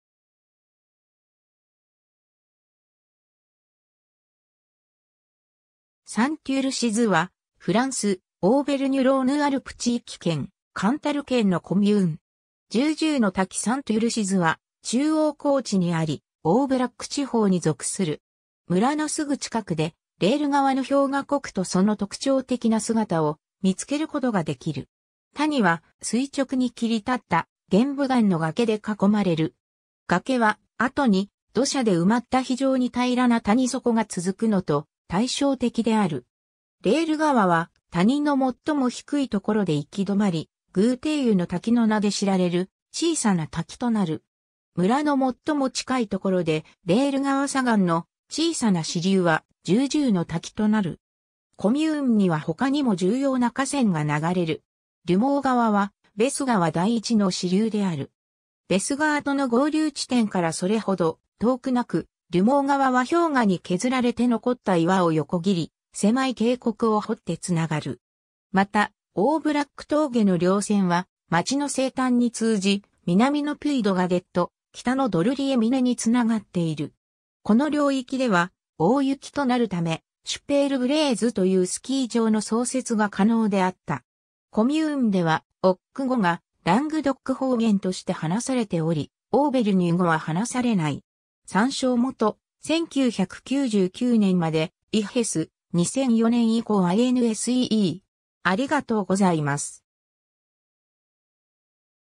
サンテュルシズ
サンテュルシズ （Saint-Urcize、オック語:Sent Rocise）は、フランス、オーヴェルニュ＝ローヌ＝アルプ地域圏、カンタル県のコミューン。
 ジュー・ジューの滝 サンテュルシズは、中央高地にあり、オーブラック地方に属する。
村のすぐ近くで、レール川の氷河谷とその特徴的な姿を見つけることができる。
谷は、垂直に切り立った玄武岩の崖で囲まれる。
崖は、後に土砂で埋まった非常に平らな谷底が続くのと対照的である。
レール川は谷の最も低いところで行き止まり、グーテイユの滝の名で知られる小さな滝となる。
村の最も近いところで、レール川左岸の小さな支流はジュー・ジューの滝となる。
 コミューンには他にも重要な河川が流れる。
リュモー川はベス川第一の支流である。
ベス川との合流地点からそれほど遠くなく、リュモー川は氷河に削られて残った岩を横切り、狭い渓谷を掘ってつながる。
 また、オーブラック峠の稜線は町の西端に通じ、南のピュイ・ド・ガデット（1427m）、北のドルリエ峰（1342m）につながっている。
この領域では大雪となるため、シュペール・ブレーズというスキー場の創設が可能であった。
 コミューンではオック語がラングドック方言として話されており、オーヴェルニュ語は話されない。
 参照元:1999年までEHESS、2004年以降INSEE